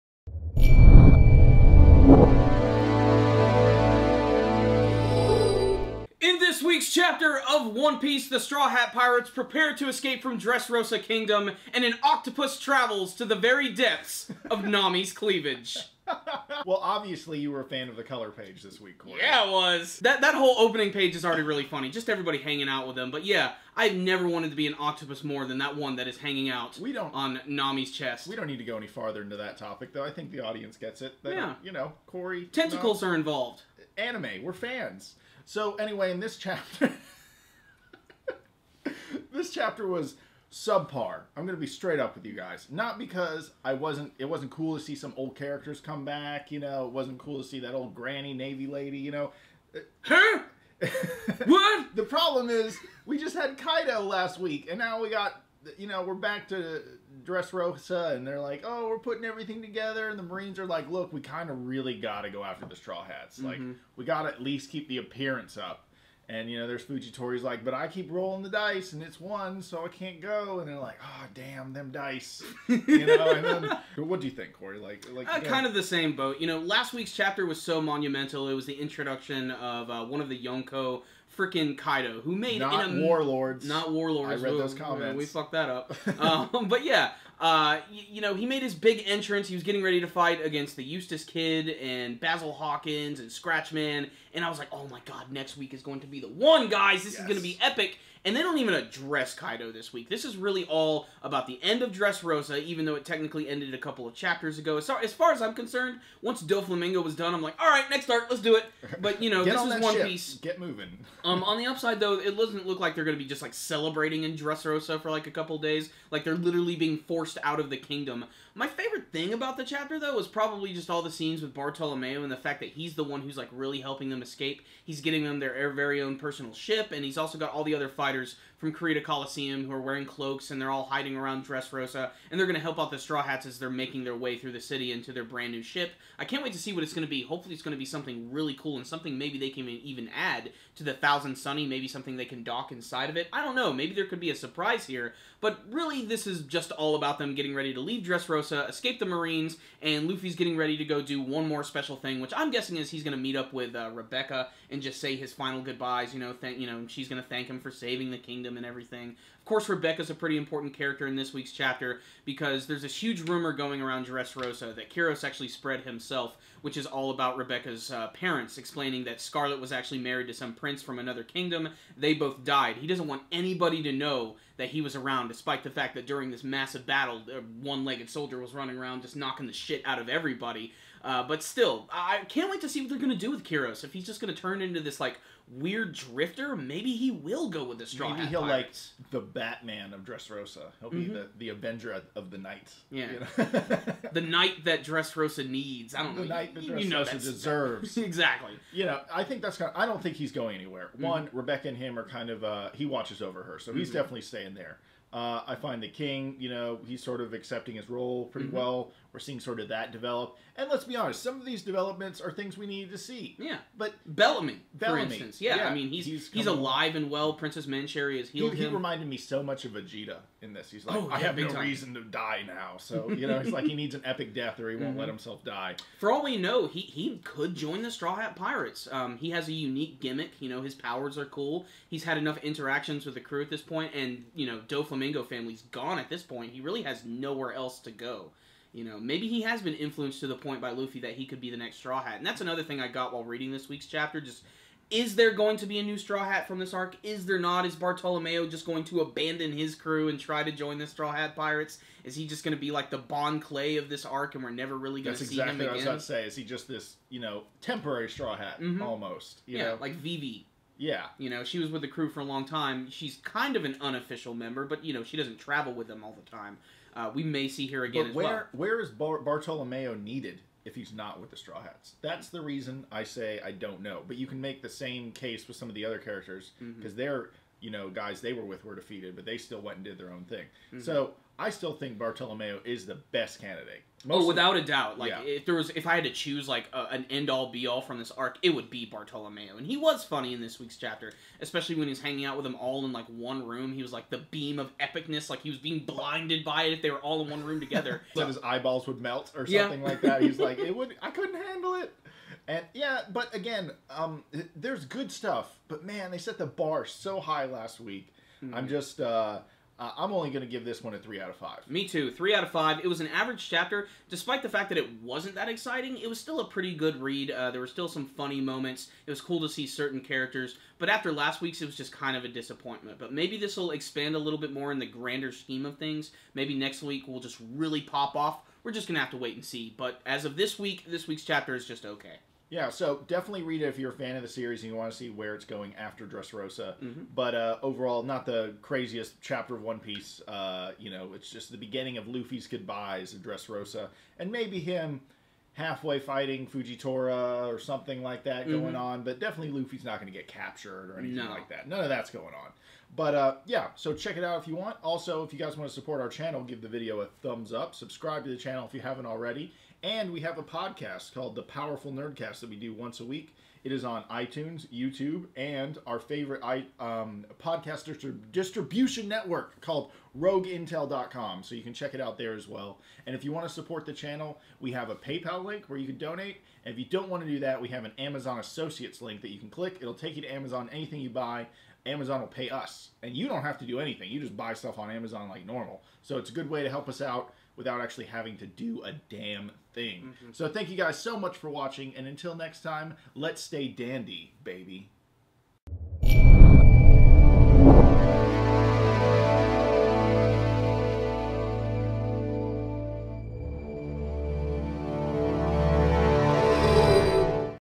One Piece 796 Manga Chapter Review ワンピース - Escape From Dressrosa!
The Straw hats prepare to escape the now marine filled Dressrosa kingdom! Did you like this weeks chapter of One Piece?
Check out our show on Itunes and at Rogue Intel, you can download the show for free!